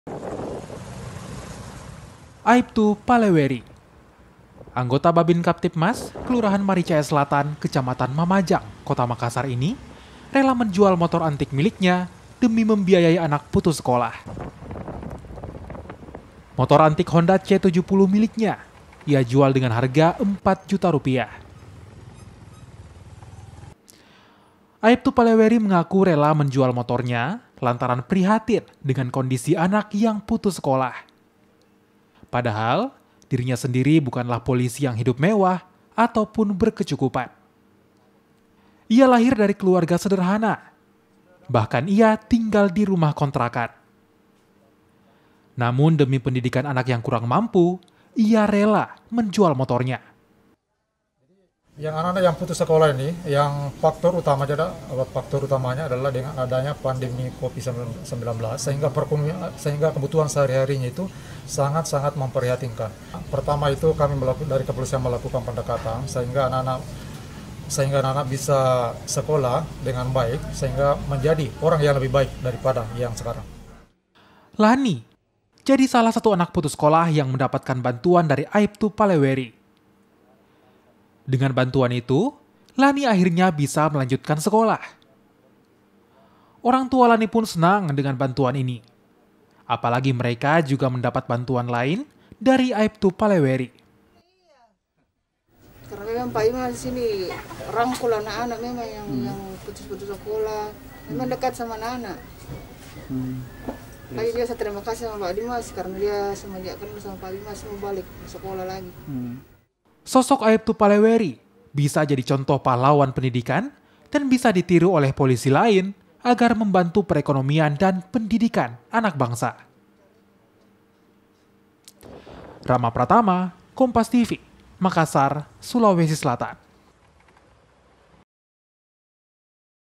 Polisi Jual Motor Untuk Bantu Anak Putus Sekolah
MAKASSAR, KOMPAS.TV - Anggota polisi di kota makassar  sulawesi selatan  rela menjual motor antik miliknya demi membantu anak putus sekolah akibat pandemi corona . Selain memberikan biaya sekolah  anggota polisi ini juga membantu keluarga anak putus sekolah .

Aiptu palewari  anggota bhabinkamtibmas kelurahan maricaya selatan  kecamatan mamajang  kota makassar  rela menjual motor antik miliknya demi membiayai anak putus sekolah .

Motor antik honda c70 miliknya ia jual dengan harga 4 juta rupiah . Aiptu palewari mengaku rela menjual motornya lantaran prihatin dengan kondisi anak yang putus sekolah . Padahal  dirinya sendiri bukanlah polisi yang hidup mewah atau berkecukupan .

Ia lahir dari keluarga sederhana  bahkan ia tinggal di rumah kontrakan . Namun  demi pendidikan anak kurang mampu, ia rela menjual motornya .

Lani jadi salah satu anak putus sekolah yang mendapatkan bantuan dari aiptu palewari . Dengan bantuan itu  lani  akhirnya bisa menaljutkan sekolah . Orang tua lani pun senang dengan bantuan ini  apalagi mereka juga mendapat bantuan lain dari aipu palewari .

Sosok aiptu palewari  bisa jadi contoh pahlawan pendidikan dan bisa di tiru oleh pollisi lain agar membantu perekonomian dan pendidikan anak bangsa .